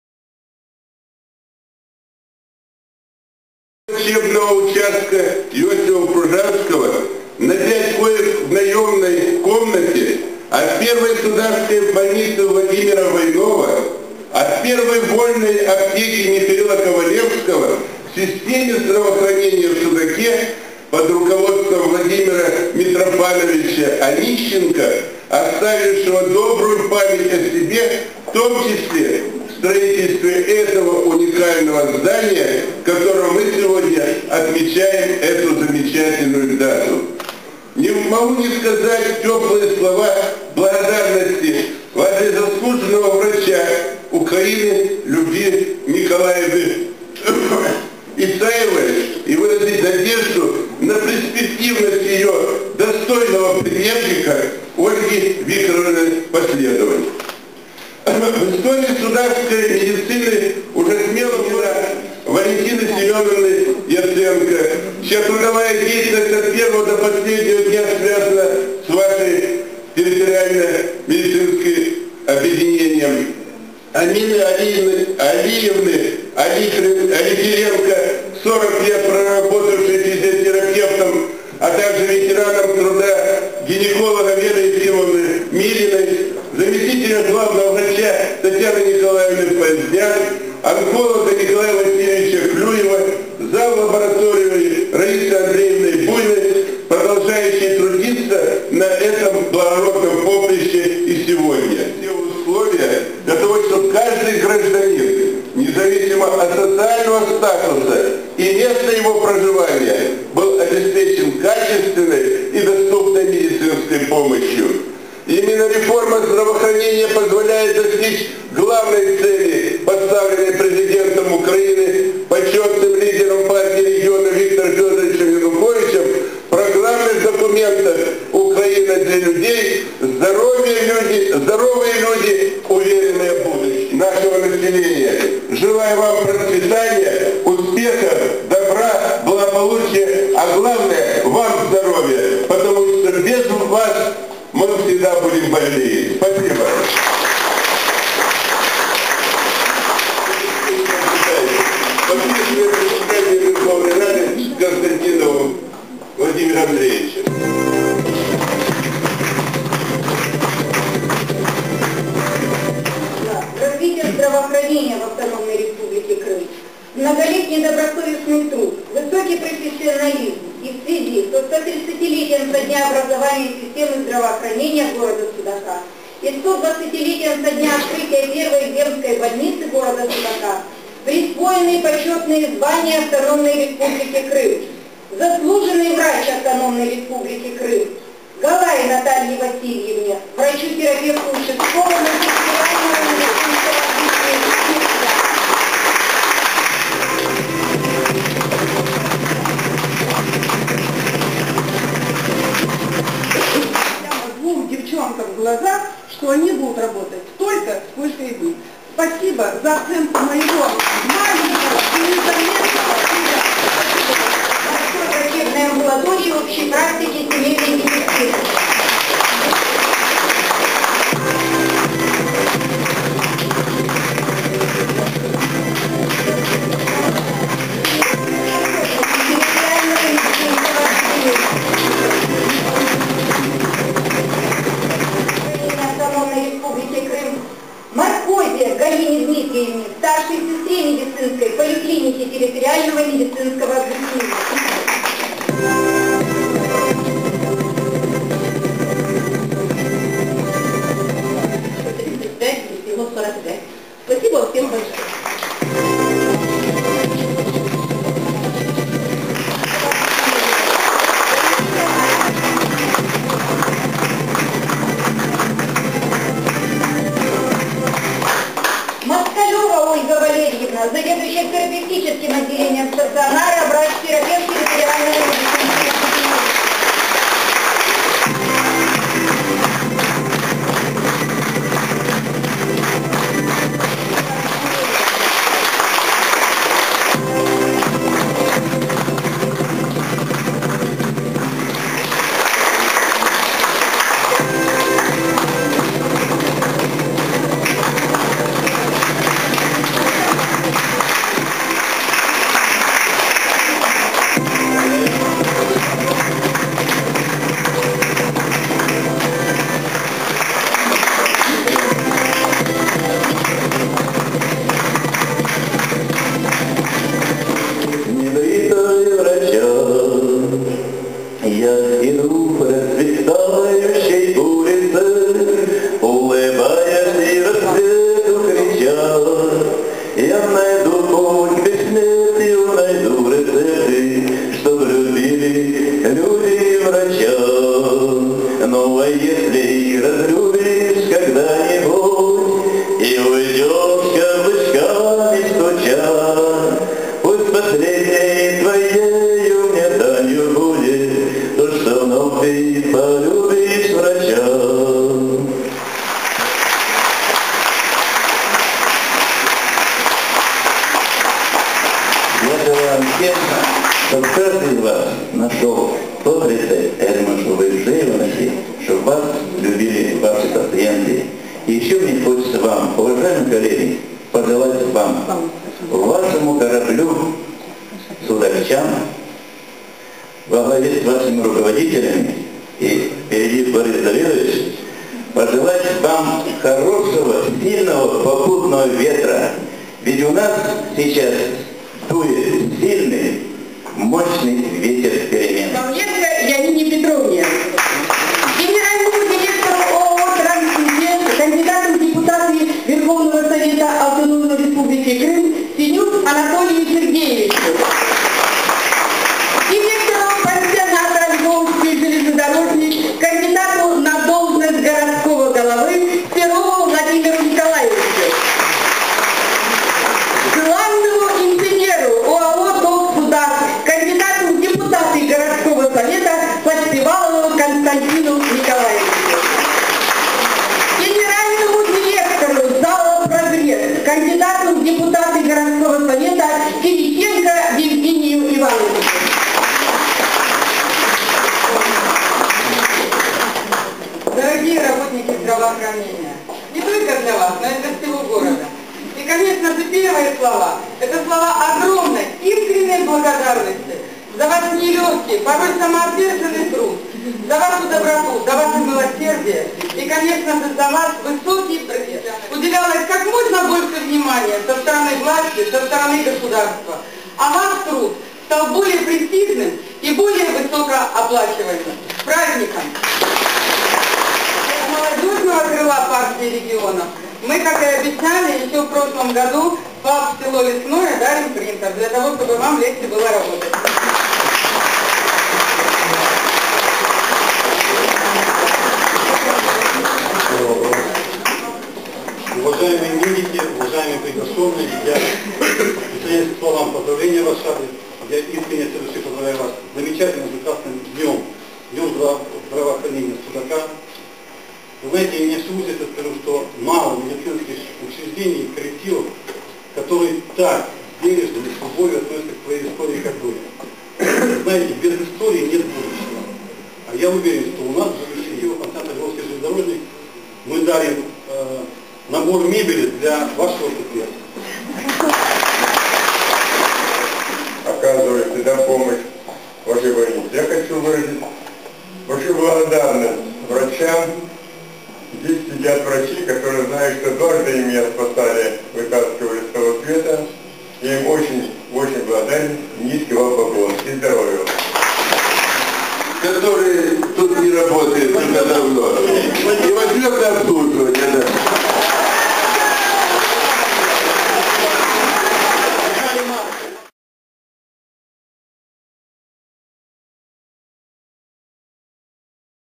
130 лет службе здравоохранения в Судаке.wmv
Празднование "130-ти летия службы здравоохранения" в Судаке. Крымский репортаж